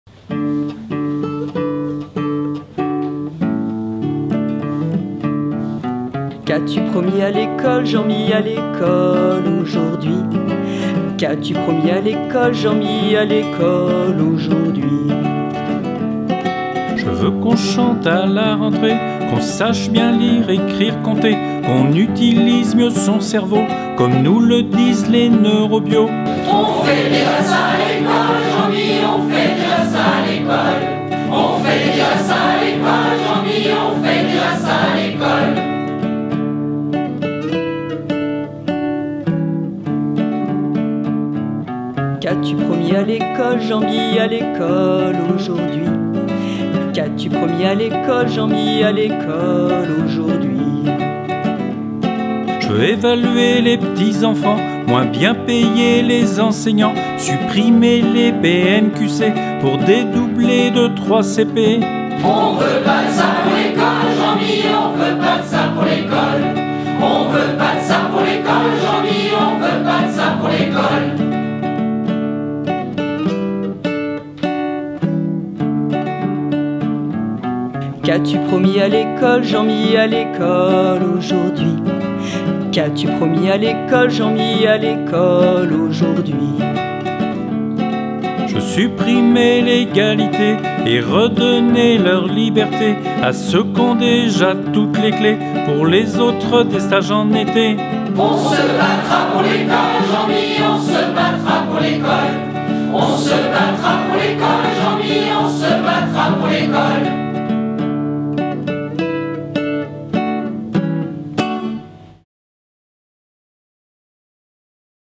Clin d’œil du SNUipp-FSU pour une rentrée en musique
En cette rentrée, le ministre invite à chanter les élèves et les enseignants. Le SNUipp-FSU, qui connaît la musique, a donc puisé dans son répertoire une chanson qui parle de l'école... en prenant quelques libertés avec les paroles originales pour prendre le contre-pied des mauvaises rengaines de la rue de Grenelle.